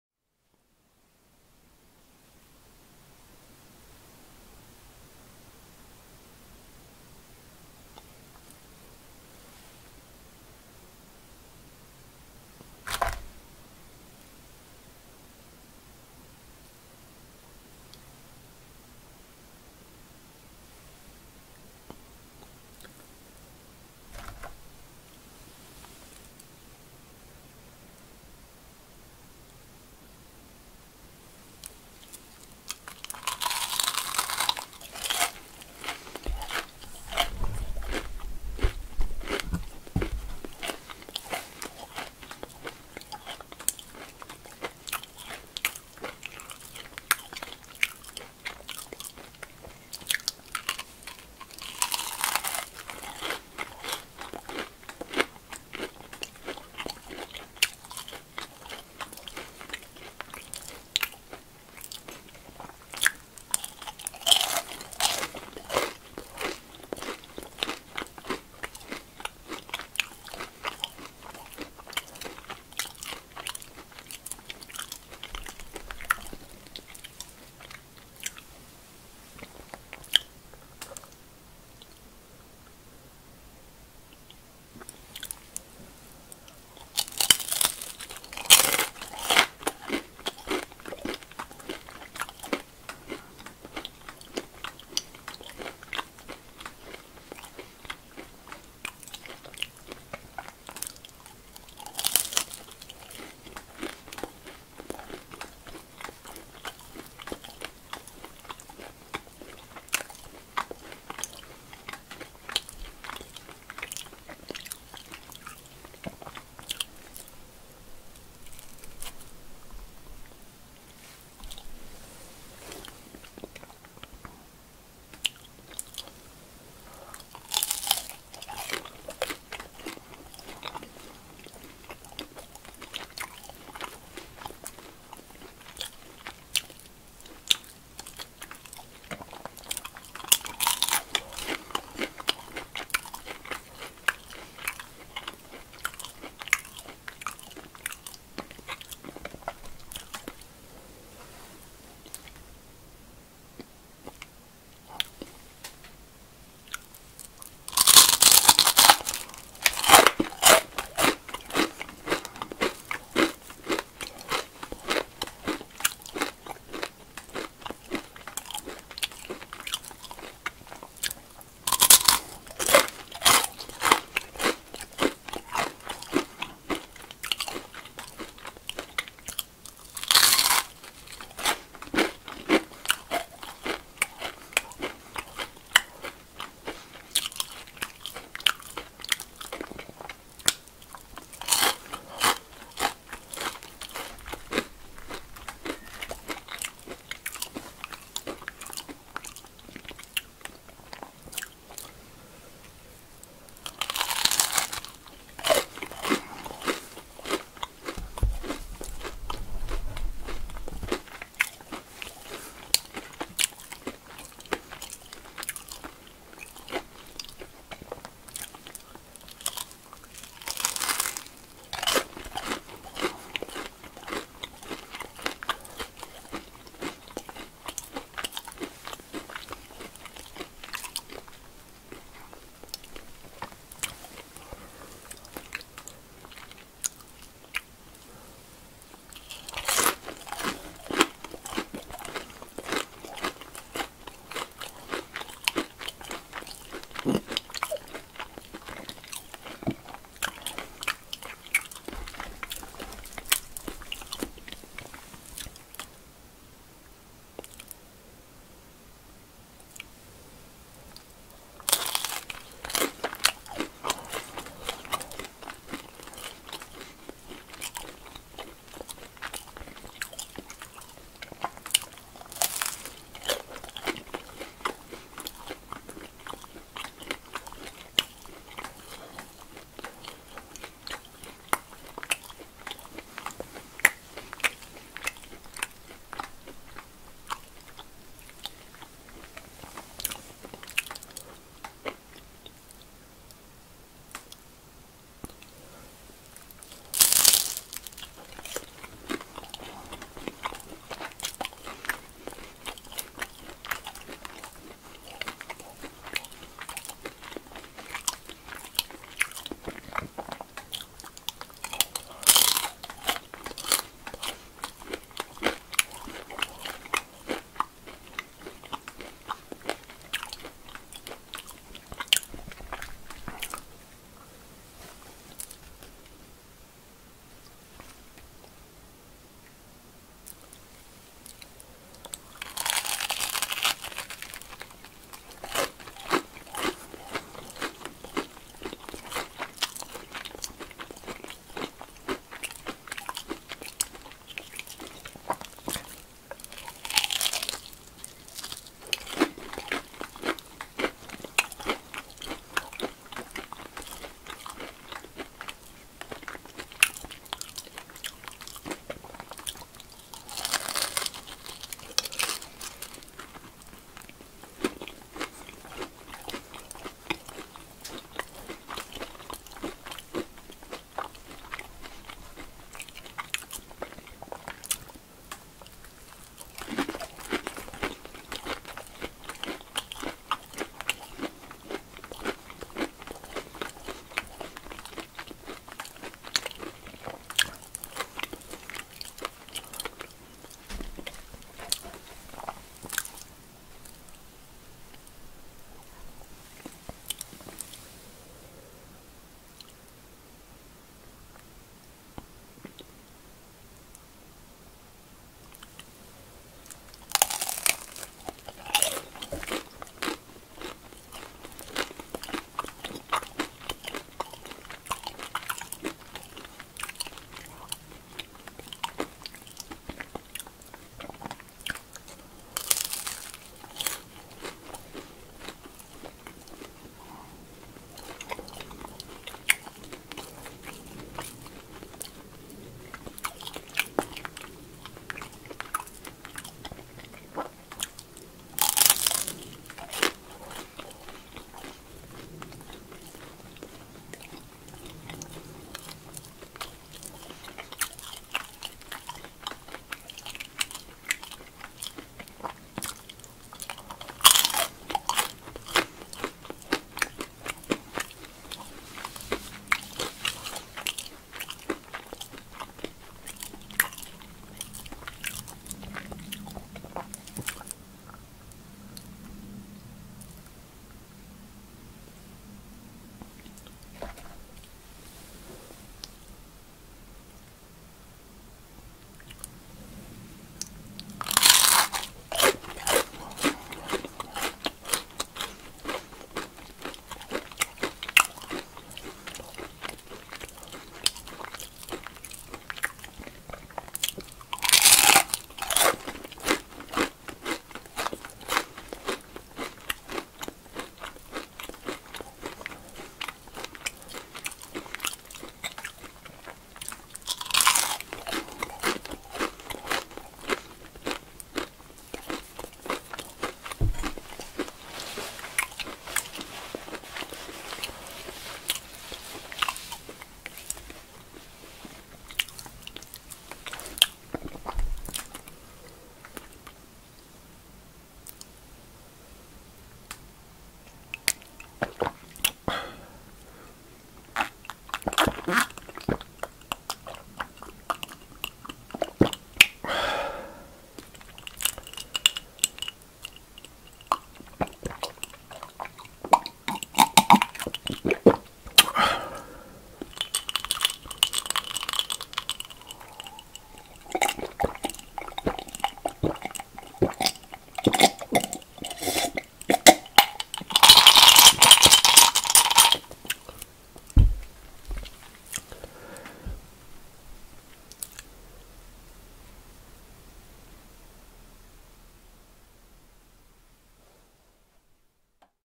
ASMR CRUNCHY Eating Sounds of ONION RINGS | TAQUITOS | CHEESE SAUCE. NO TALKING ! EATING SOUNDS !💤
ASMR Eating CRUNCHY FOOD.  ASMR EATING SOUNDS ONLY !  Eating Show.  ASMR NO TALKING !
Eating CRUNCHY ONION RINGS | CRUNCHY TAQUITOS | and CHEESE SAUCE. ASMR Eating Sounds. Sounds to Relax You and give you Tingles and Sleep.💤 🔊( Offered In High Quality Sound!!!